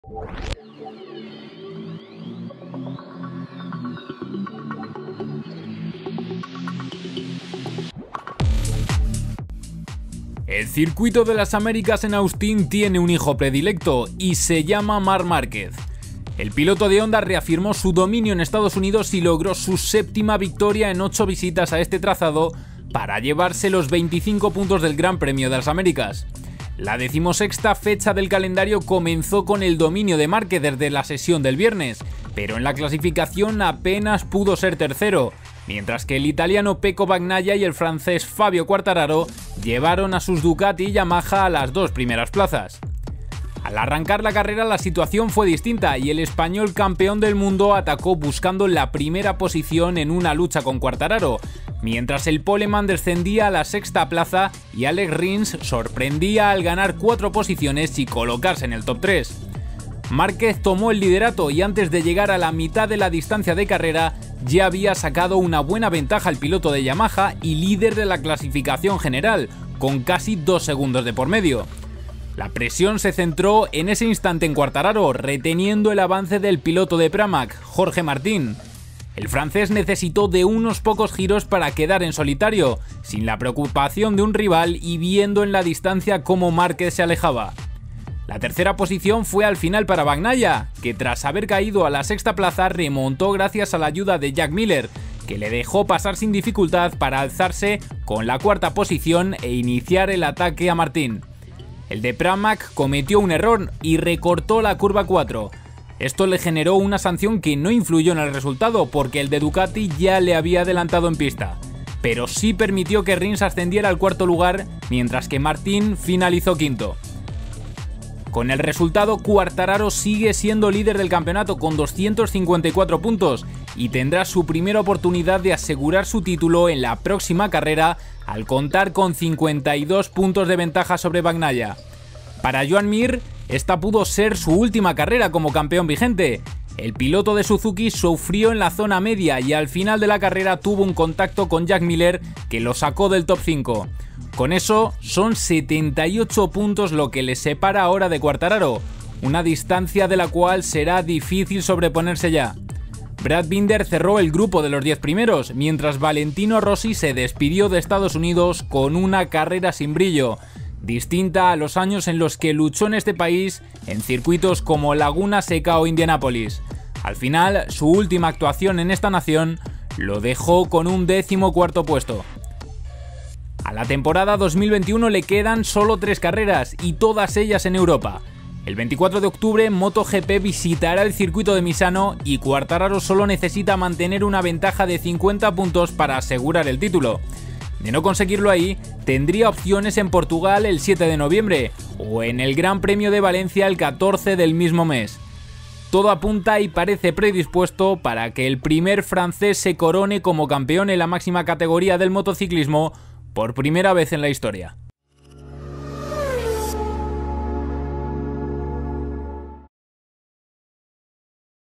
El resumen de la carrera de MotoGP del GP de las Américas 2021
Marc Márquez consiguió otra aplastante victoria en el Circuito de las Américas y vuelve a ser el sheriff de Austin. Fabio Quartararo, que fue segundo, tiene a tiro el título de MotoGP, mientras que el vigente campeón Joan Mir fue sancionado por una acción con Jack Miller.